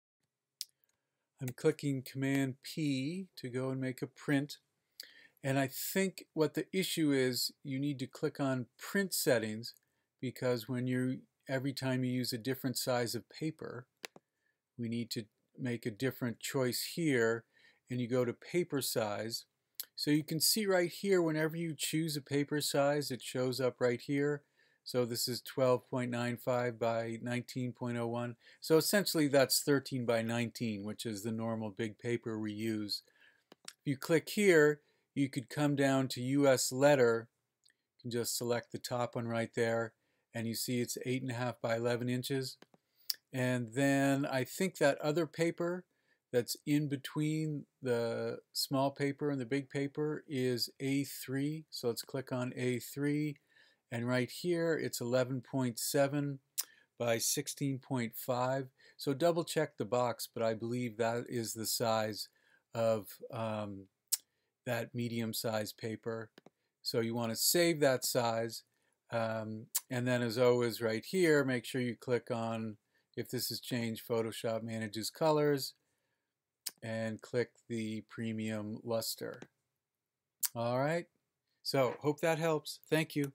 How to Select the Proper Paper Size In Photoshop
Do you like to express your ideas visually?  Professor Nordell teaches courses in the Visual and Digital Arts program at American International College in Springfield, MA.